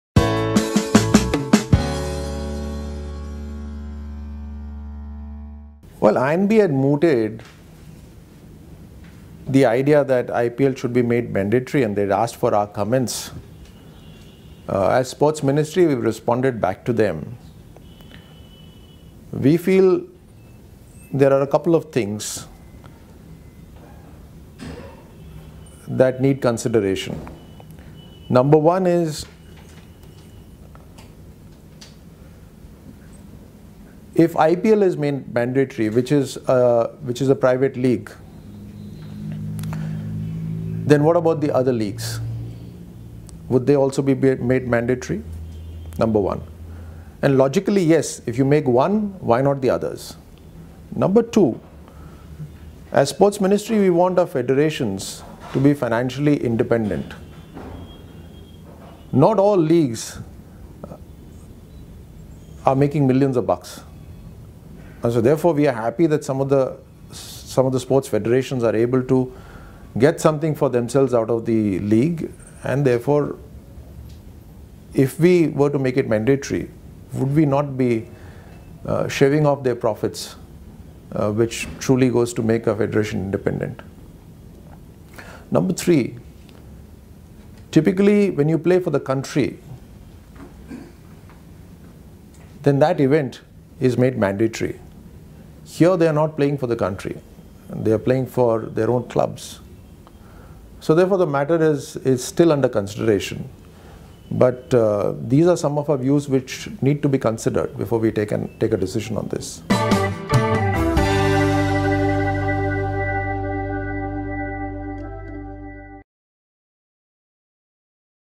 Sports Minister Rajyavardhan Singh Rathore On The Idea Of Broadcasting IPL Matches On Doordarshan
Since the inception of the IPL, private television broadcasters have been able to bring in huge amounts of viewership and profit. Although the government broadcasters have been missing out on this opportunity, recently there have been talks on the idea of broadcasting IPL matches on Doordarshan. In In an interactive session at Idea Exchange by the Indian Express, Minister Of State, Rajyavardhan Singh Rathore shared his view on this.